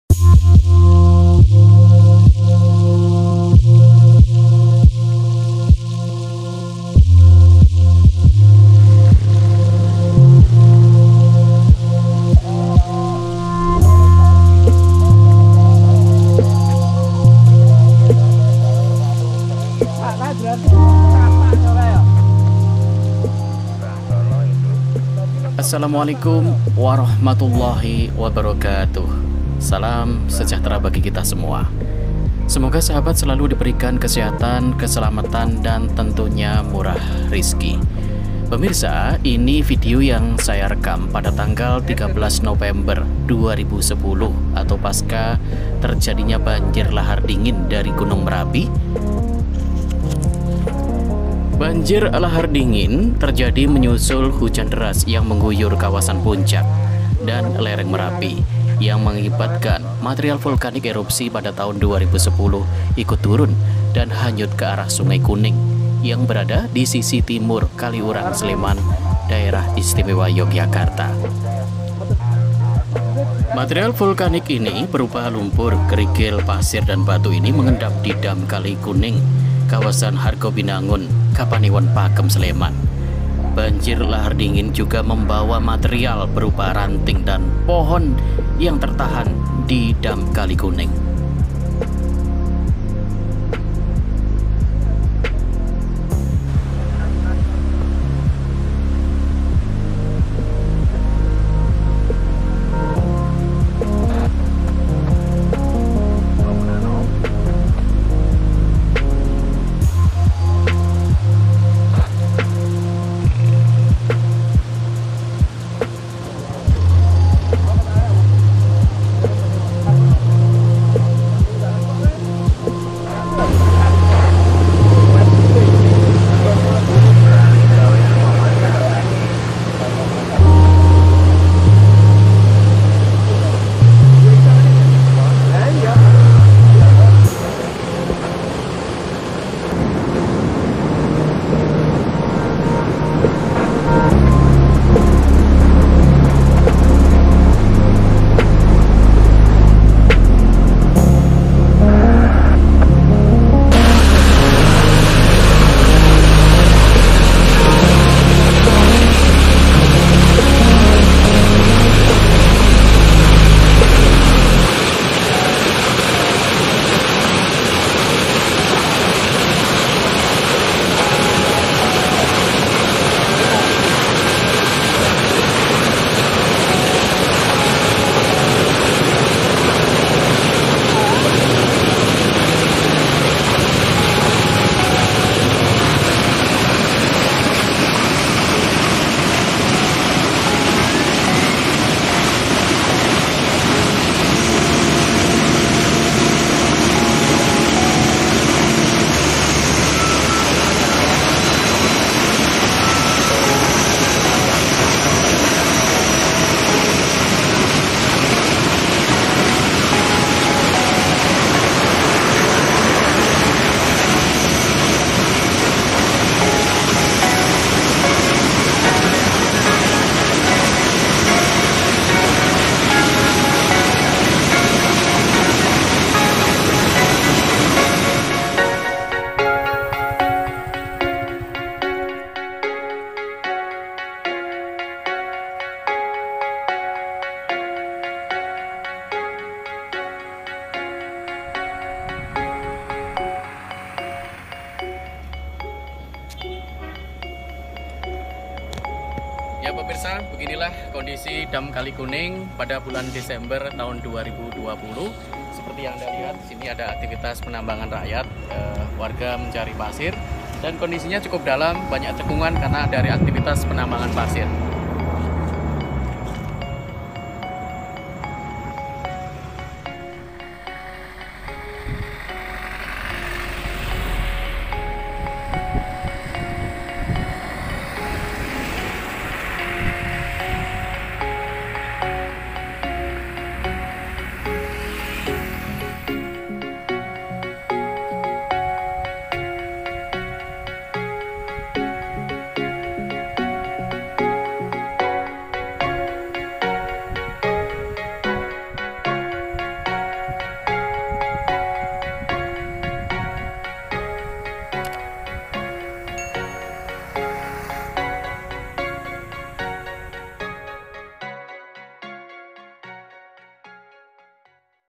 Banjir lahar dingin merapi sungai kuning (erupsi 2010)
kondisi sungai kuning Pakem Sleman Yogyakarta pasca banjir lahar dingin merapi tgl 13 Nopember 2010